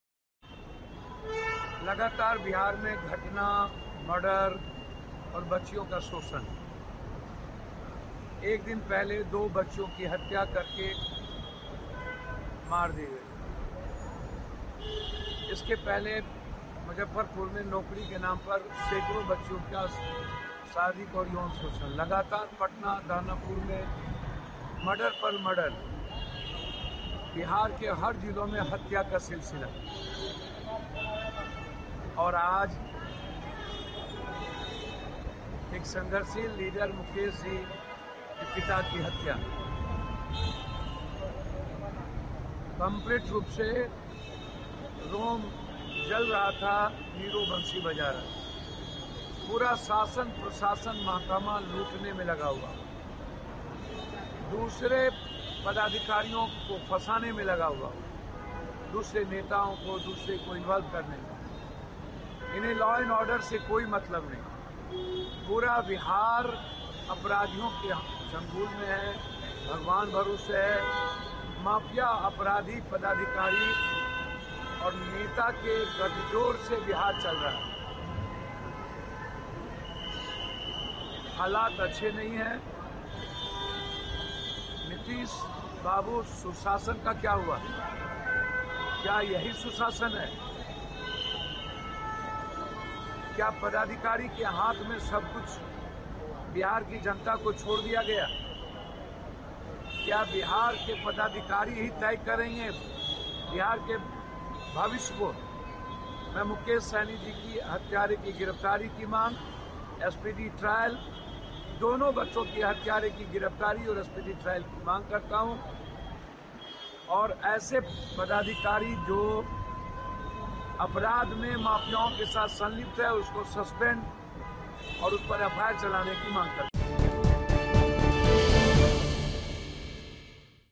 Purnia सांसद Pappu Yadav ने Nitish Kumar पर साधा निशाना
मुकेश सहनी के पिता की हत्या के बाद पूर्णिया सांसद पप्पू यादव ने बिहार सरकार पर कहा कि "लगातार बिहार में घटना, मर्डर और बच्चियों का शोषण हो रहा है। लगातार पटना दानापुर में मर्डर पर मर्डर हो रहे हैं, बिहार के हर जिलों में हत्या का सिलसिला चालू है और आज मुकेश सहनी के पिता की हत्या हो गई है। पूरा शासन, प्रशासन, महकमा लूटने में लगा हुआ है। पूरा बिहार अपराधियों के चंगुल में है, भगवान भरोसे है। माफिया, पदाधिकारी और नेता के गठजोड़ से बिहार चल रहा है। "